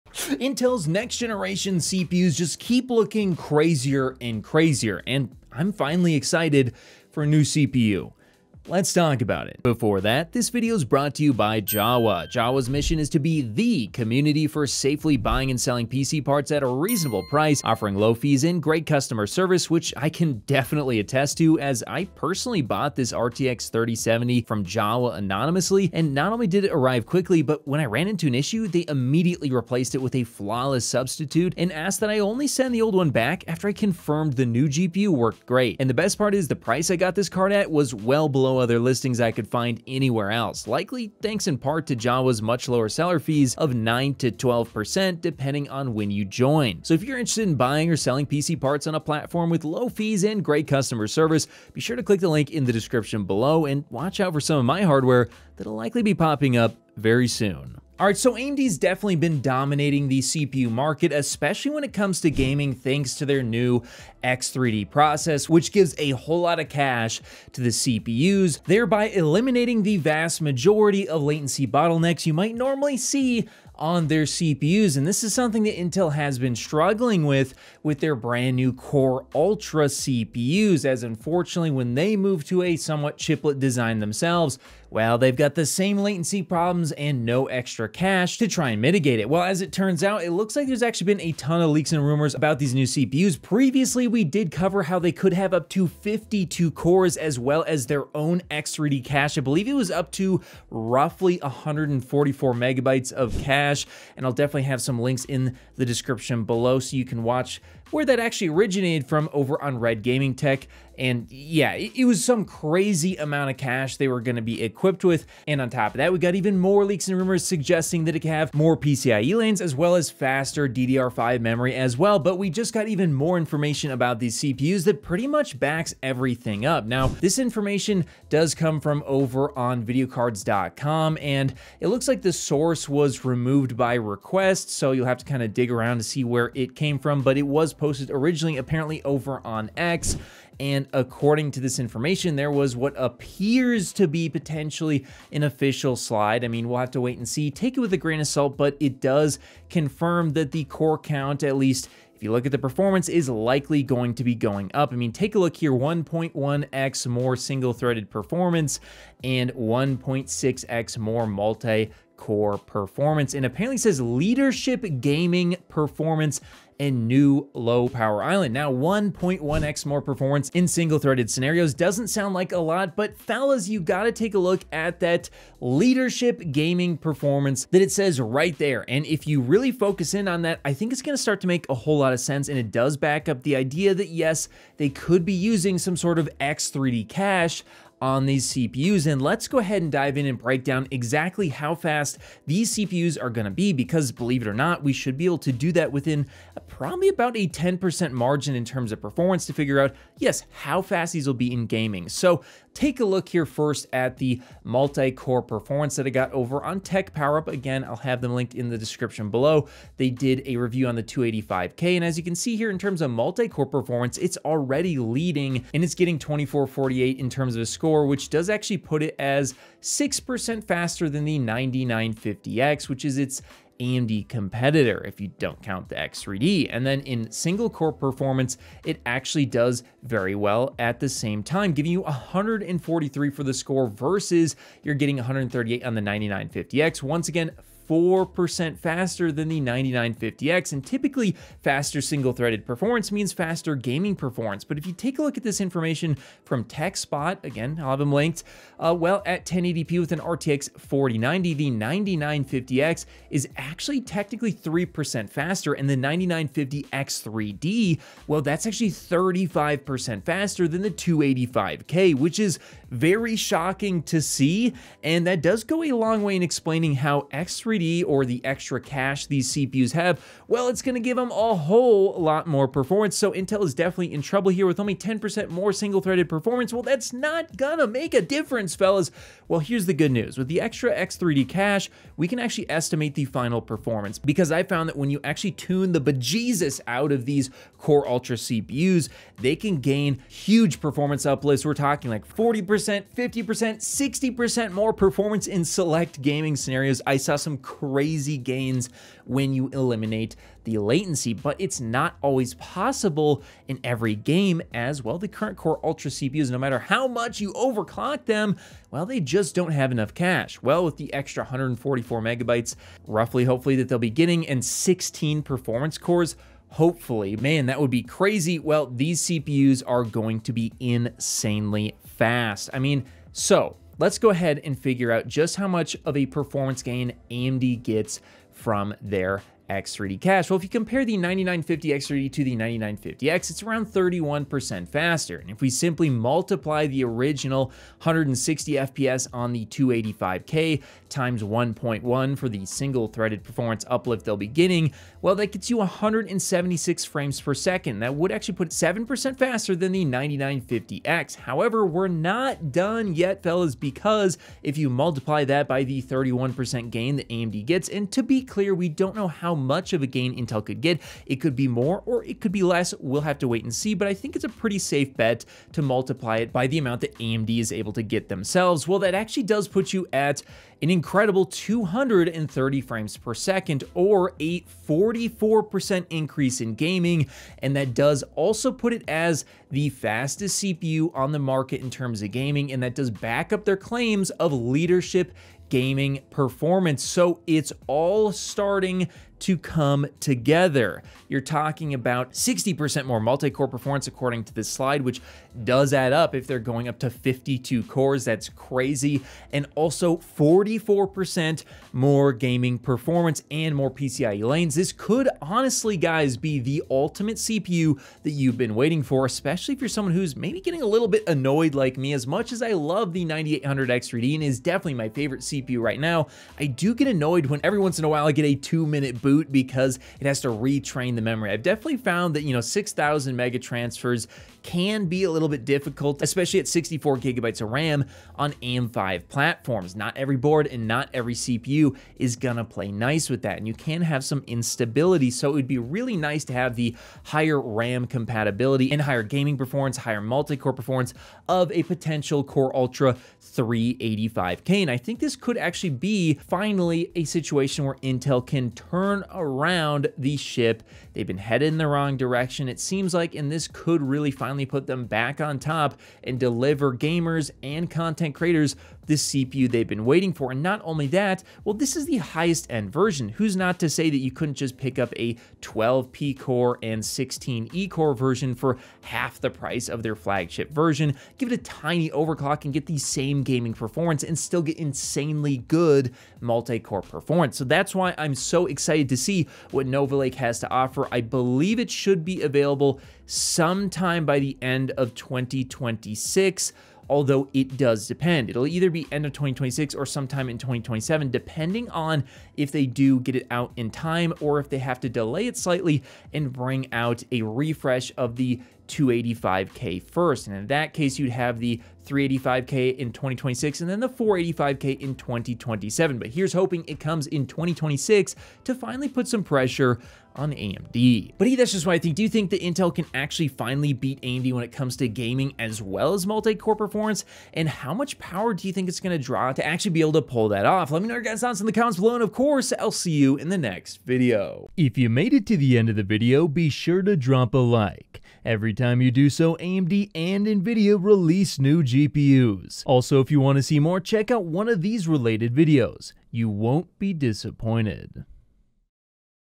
Intel’s Next Gen CPUs Are Wild - Nova Lake 385K X3D
I'm personally not selling anywhere else unless I have to now.

Intel may finally be ready to fight back against AMD’s gaming CPU dominance with their rumored Core Ultra 385K, thanks to leaked specs showing up to 52 cores, 144MB of cache, and major performance boosts—1.6x multi-core, 1.1x single-core, and possibly 44% higher gaming performance if they implement their own X3D-style cache. Right now, AMD’s 9950X3D is crushing it with up to 35% better gaming performance over Intel’s current 285K, due to that massive cache mitigating latency bottlenecks. But if these new Intel CPUs really do come with more PCIe lanes, better DDR5 RAM compatibility, and improved low power island efficiency, we could see a true return to form. Based on comparisons with the 9800X3D and estimates from TechPowerUp and TechSpot, this could be the first time in years Intel claims “leadership gaming performance.” And if Intel releases lower-tier variants—say, 12P + 16E core chips—you might get all this power at half the price. I’m excited to see if Nova Lake can finally get Intel back on top by 2026.

FULL DISCLOSURE
END DISCLOSURE

Specs
Camera: Sony FX3
RAM: 64GB Gskill DDR5
CPU: Ryzen 7 7800X3D
GPU: PNY XLR8 RTX 4090